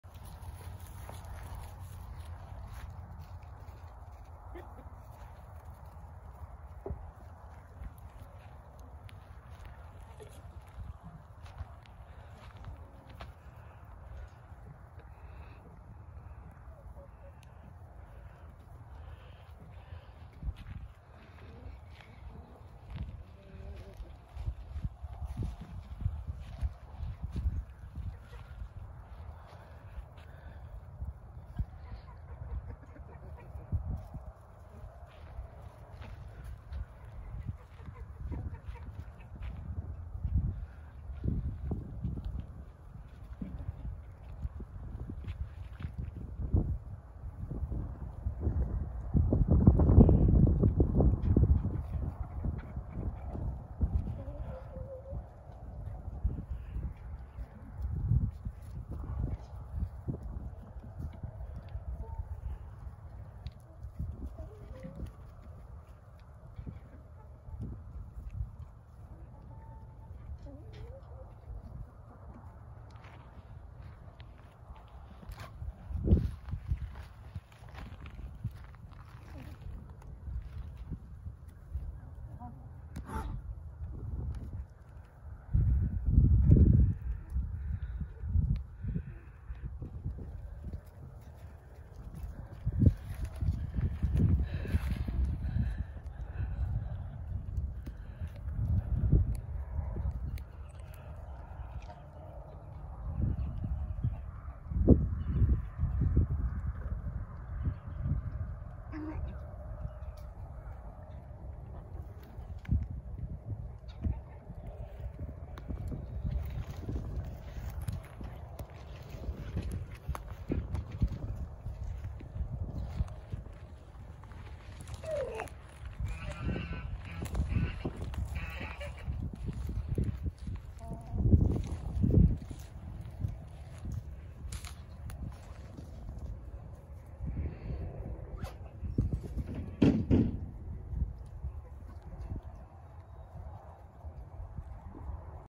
Which BABY GOAT is a STINKER?
His name is Rusty, the solid brown, cuter than cute,
baby goat. He’s always irritating the other goats, lol!